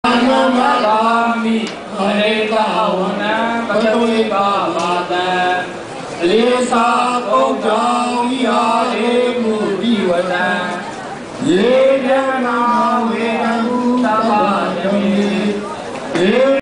Burmese Memorial Service 3
Special memorial service for those who died in the protest in Burma.